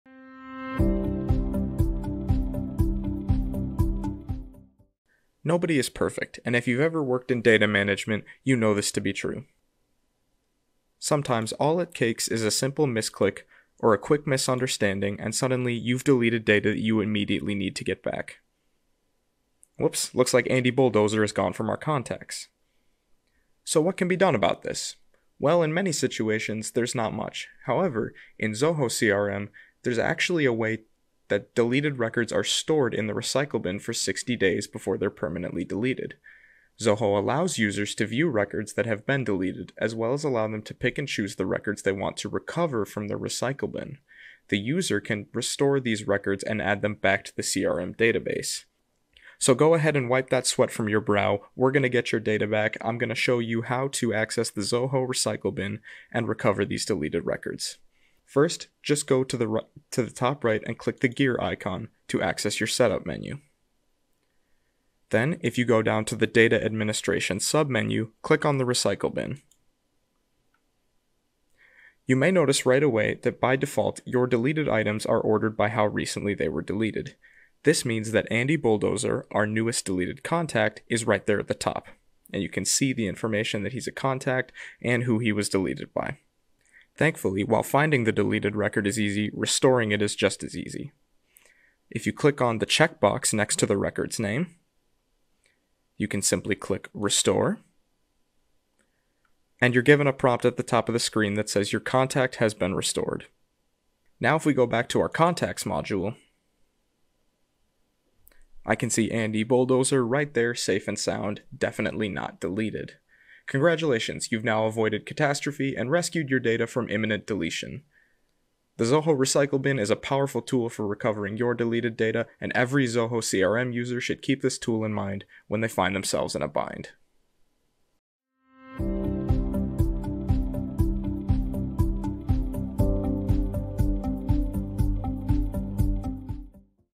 Recovering Deleted Records in Zoho CRM
No one is perfect, and that certainly holds true when it comes to data management. Everyone deletes something that they wish they had not. It may have been a simple mis-click, but that critical record is gone now, and you need it back ASAP! Luckily, Zoho CRM takes our clumsy human nature into account and has given us a safety net we can rely on to keep our data from being accidently deleted: a Recycle Bin.